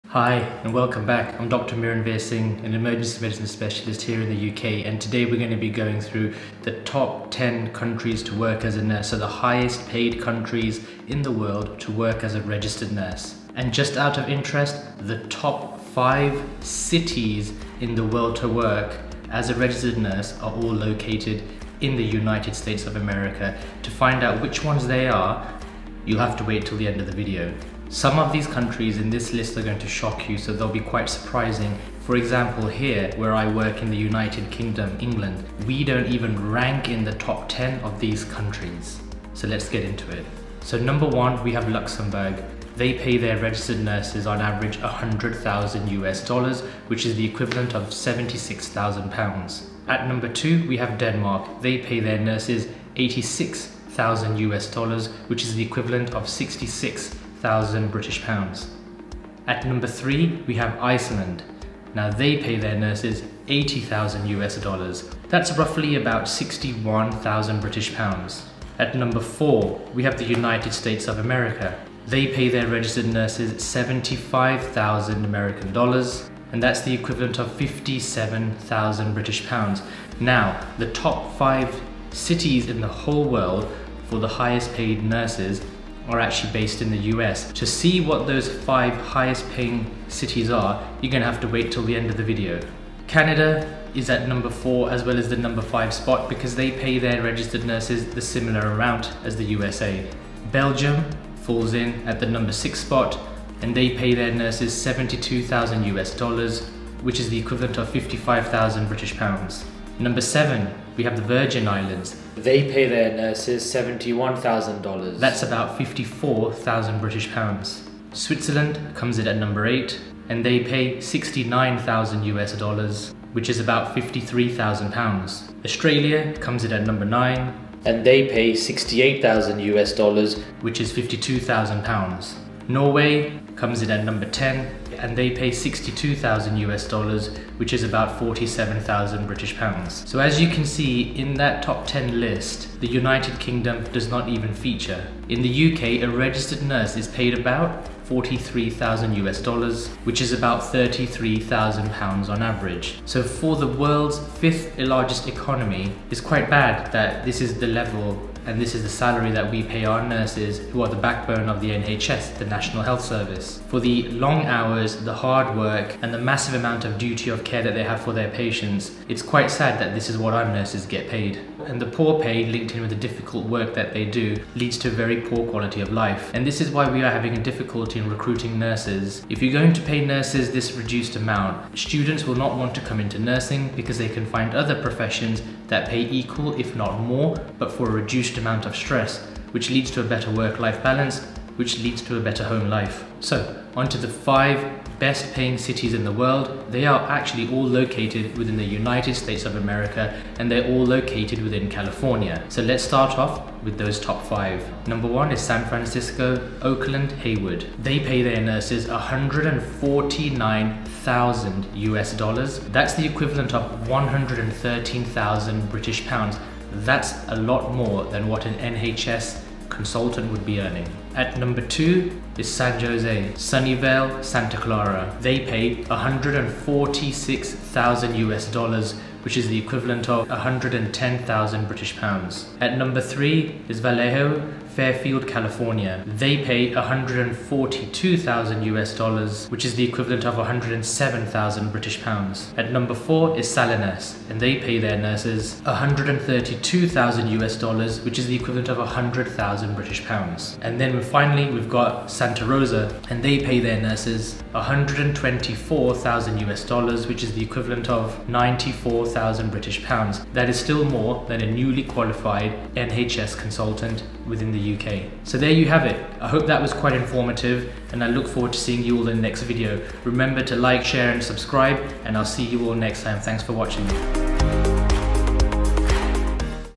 Top 10 Countries For Nurses | Highest Salaries | 2022
$149,000 highest earning is in California, USA. Top 5 are all located in California, USA, but the average earnings for the Top 10 countries for nurses to work where they have the highest salaries will surprise you.

Just a guy trying to tell a story and educate along the way.

Father
Husband
Emergency Medicine Specialist Doctor
Motivational Speaker